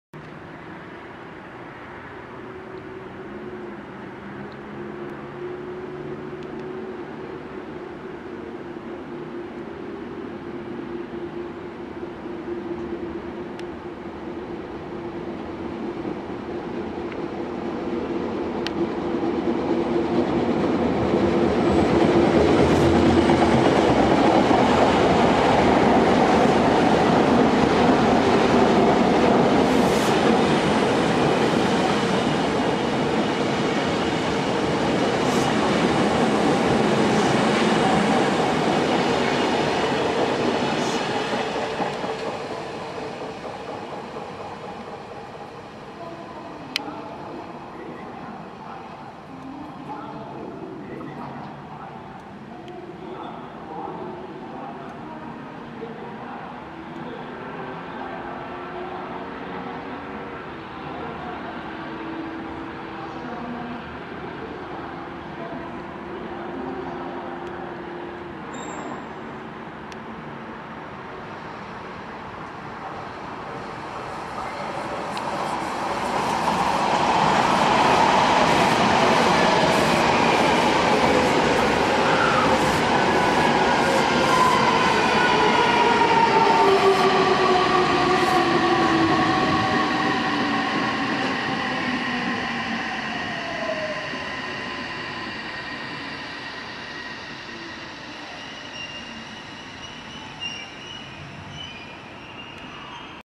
メトロもみじ号を撮っていたらEXEに被られた。 和泉多摩川
3000形(3664F)はおまけです。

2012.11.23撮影